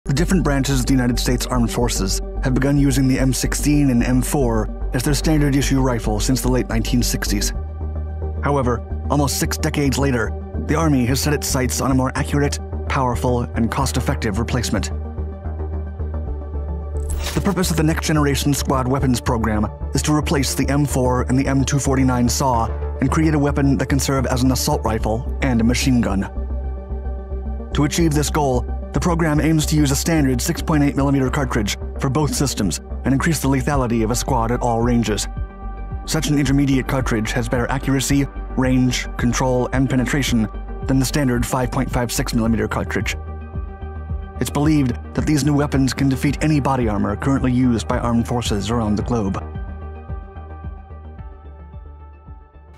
shorts Next Generation Squad Weapons Program
The different branches of the United States armed forces have been using the M16 and M4 as their standard issue rifle since the late 1960s. However, almost six decades later, the Army has set its sights on a more accurate, powerful, and cost-effective replacement.

The purpose of the Next Generation Squad Weapons Program is to replace the M4 and the M249 SAW and create a weapon that can serve as an assault rifle and a machine gun.

To achieve this goal, the program aims to use a standard 6.8-millimeter cartridge for both systems and increase the lethality of a squad at all ranges. Such an intermediate cartridge has better accuracy, range, control, and penetration than the standard 5.56-millimeter cartridge.

It is believed that these new weapons can defeat any body armor currently used by armed forces around the globe...